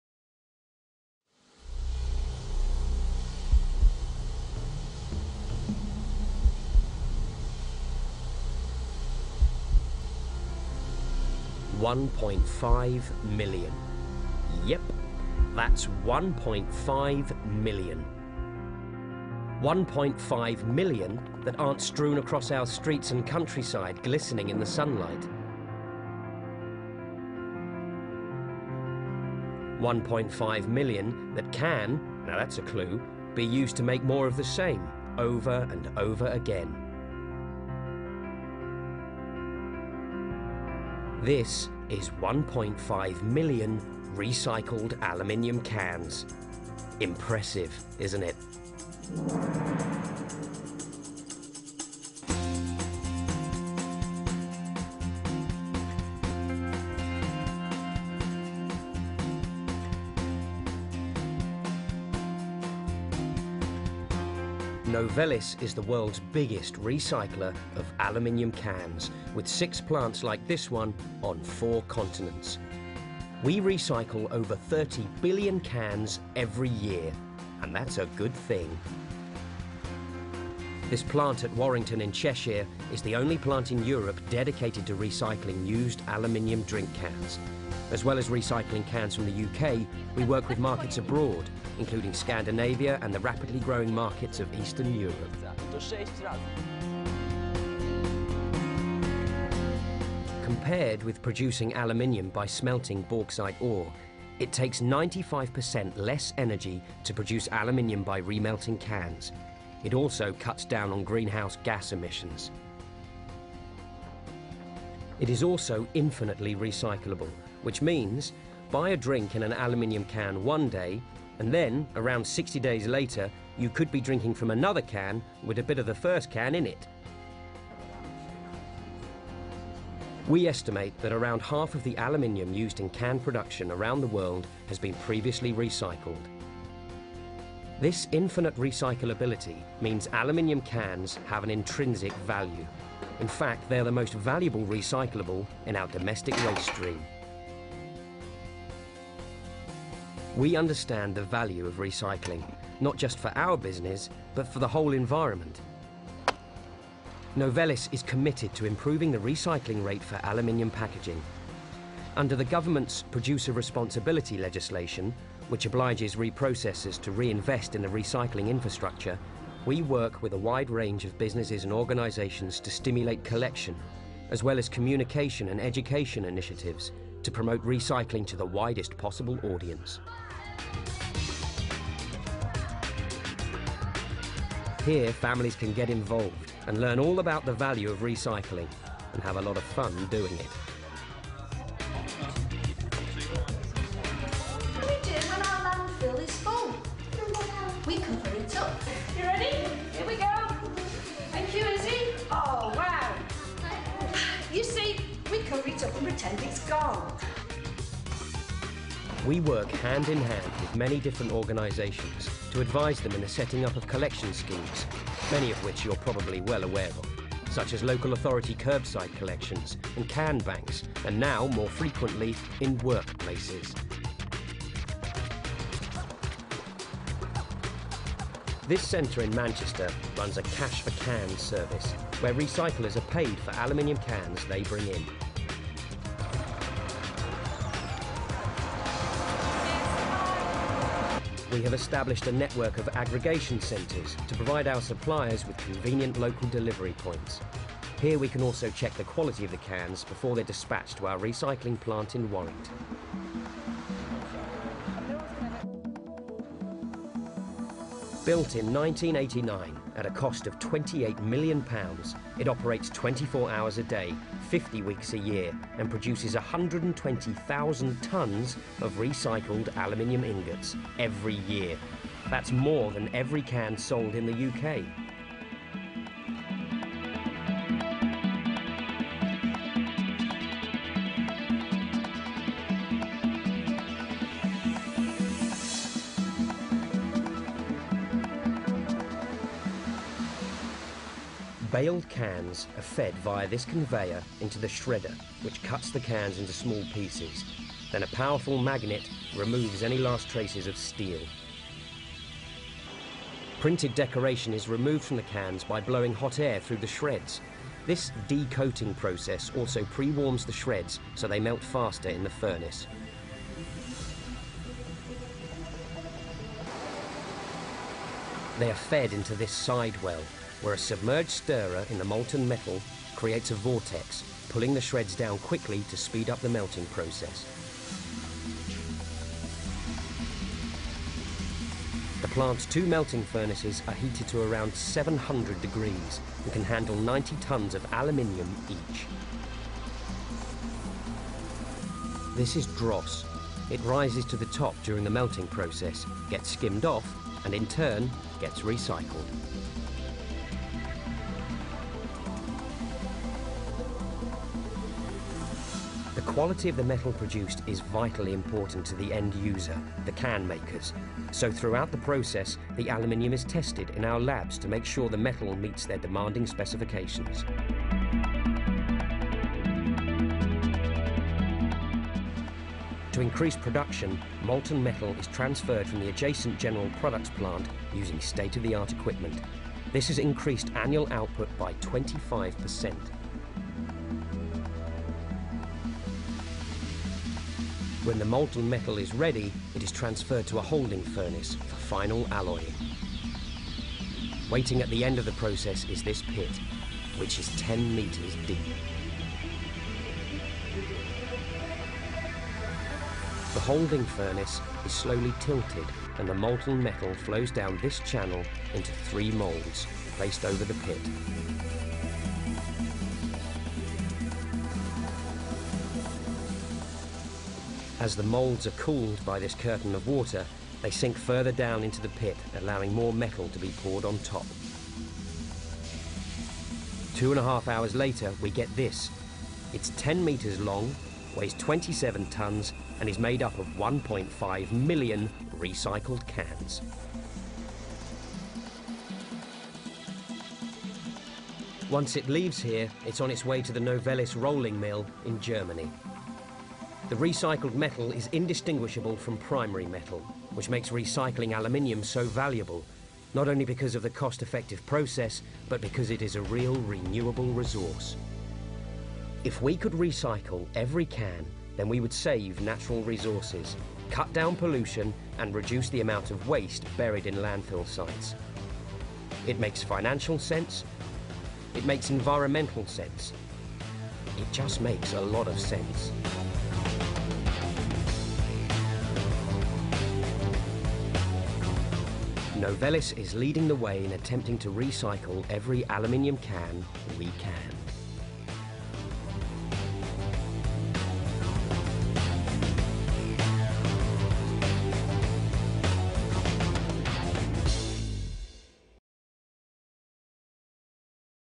Dosenrecycling Novelis Recycling
Aluminium can recycling at Novelis smelter in Warrington UK.